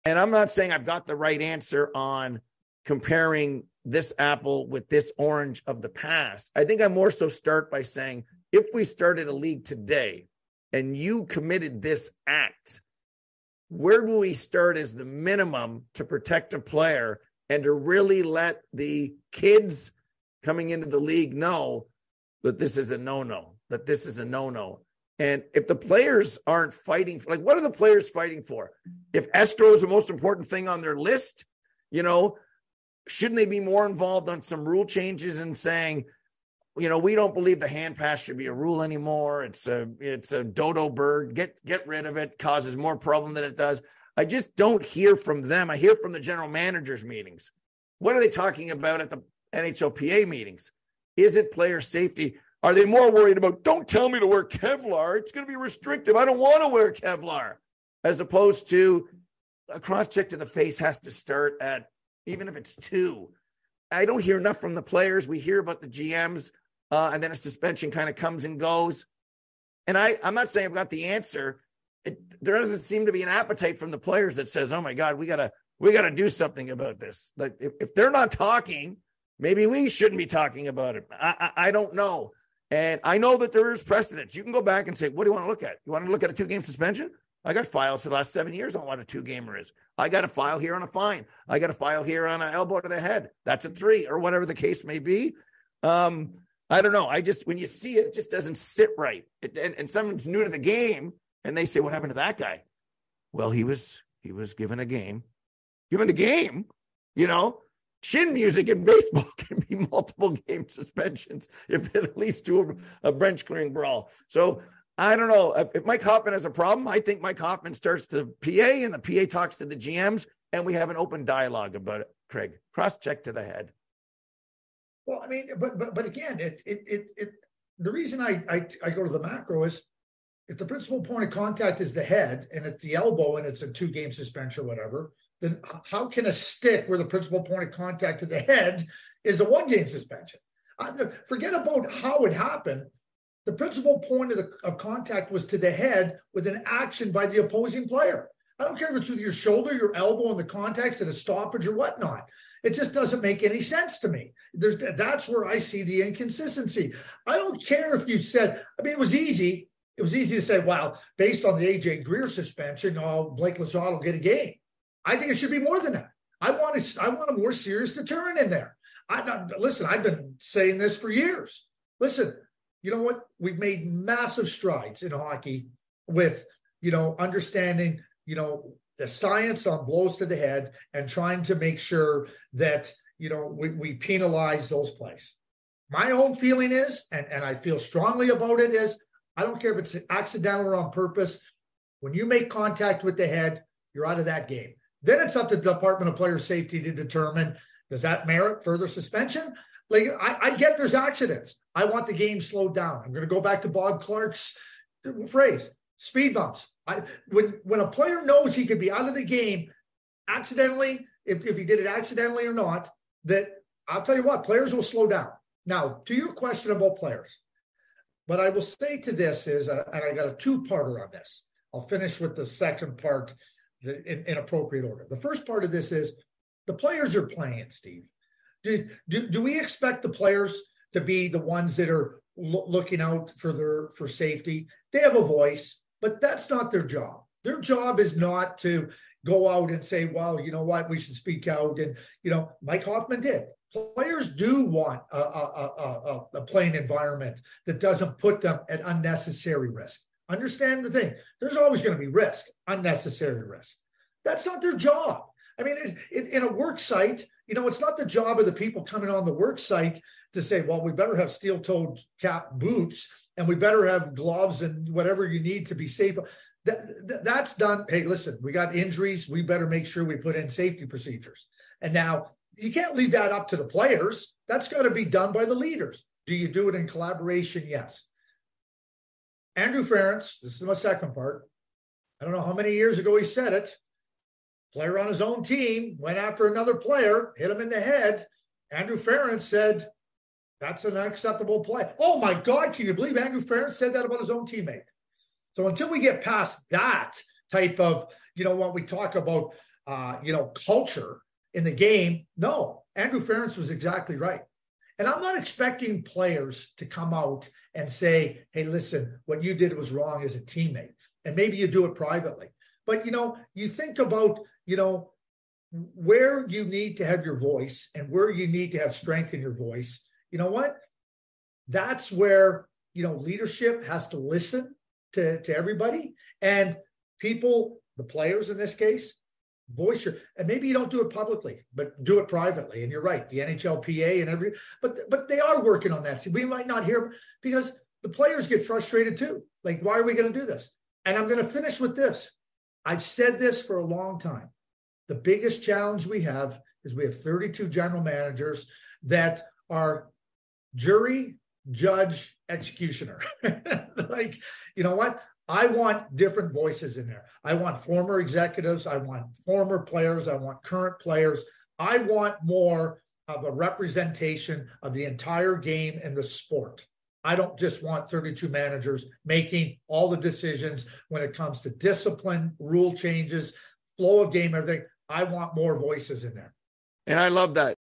Suspensions? | KBHP EP 127 - Player Safety
From our 127th Episode Steve Kouleas and Craig Button on NHL suspensions, players safety and punishments. Are we doing enough to protect players? What needs to happen?

- Consistency in discipline
- NHLPA involvement?
- Not up to the players!
- What about the victim?
- Stiffer punishments!
- Comparable in the playoffs?